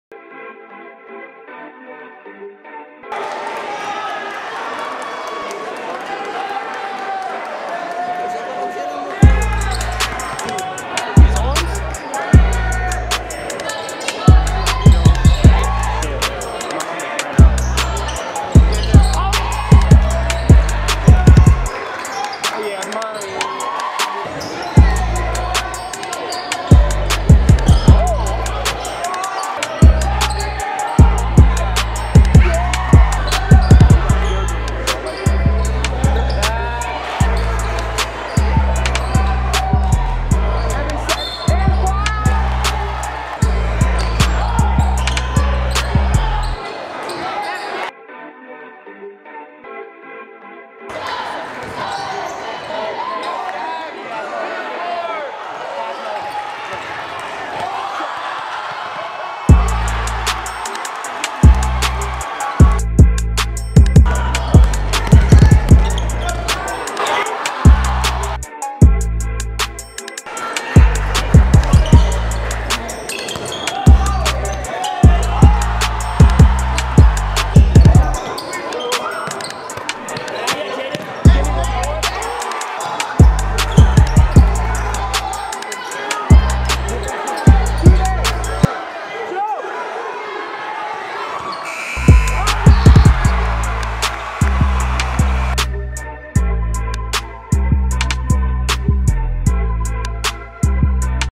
2017.2018.FP.Highlights.SrNight.21218
Watch highlights from ND basketball's win over FP on Senior Night February 12, 2018